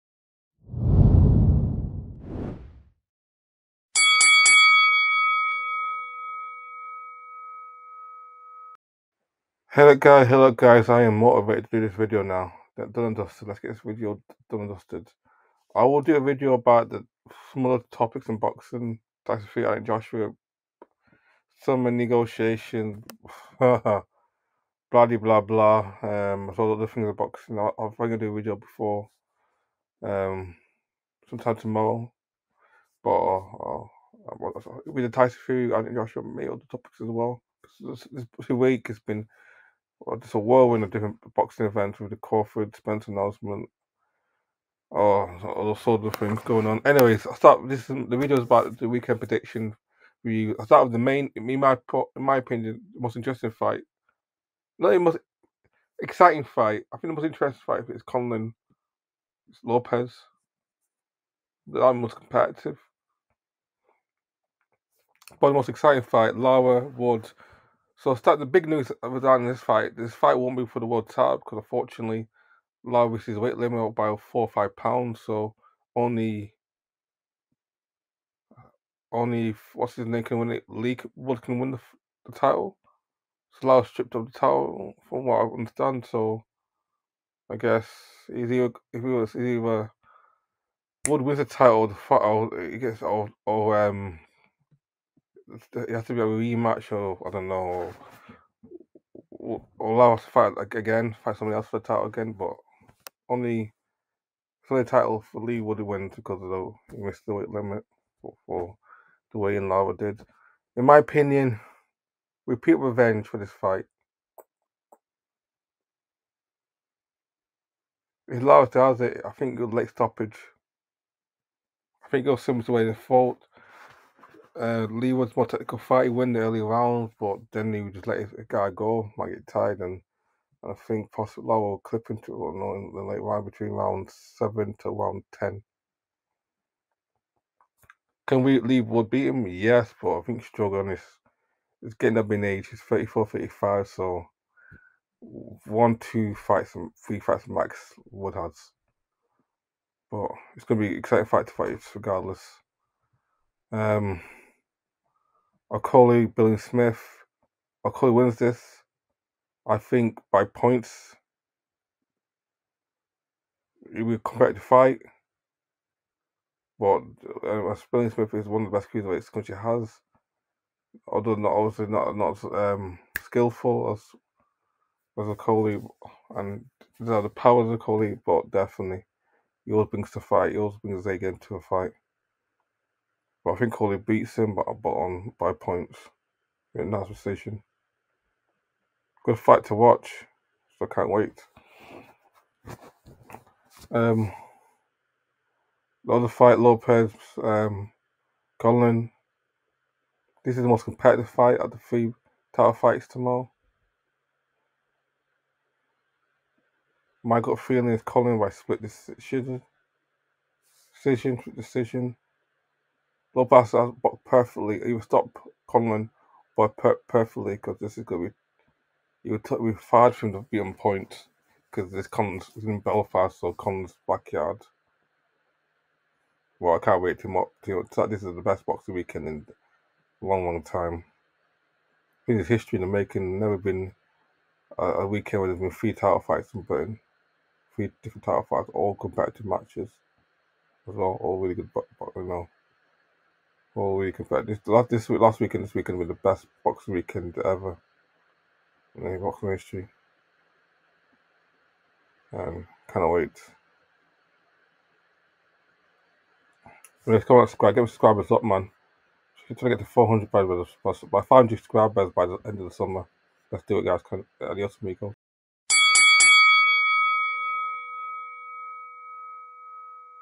A WONDERFUL WEEKEND OF PREDICTIONS! Lara vs Wood Rematch, Lopez vs Conlan & Okolie vs Biliam Smith.
Mauricio Lara vs Leigh Wood Prediction, Lopez vs Conlan & Lawrence Okolie vs Chris Billam-Smith. @boxxerofficial @skysportsboxing @TNTSportsBoxing @toprank